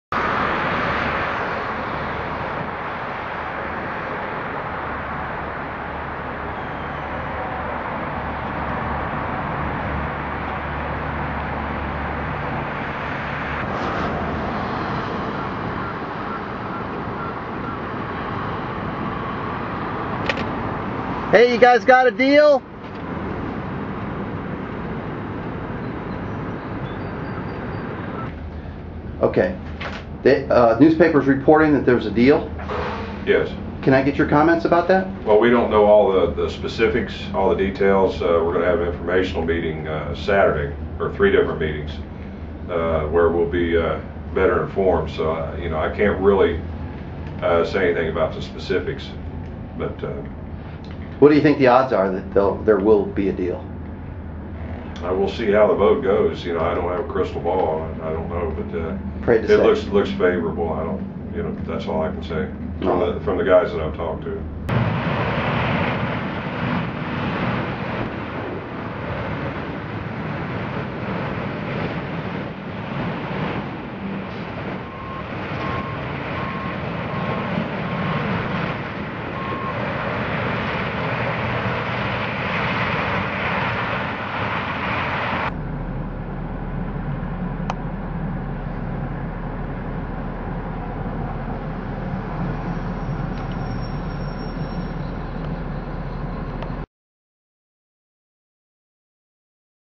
AK Steel Deal in the Works
Video of and operating AK Steel Plant and interview with one of its lockouts on the prospect of an agreement.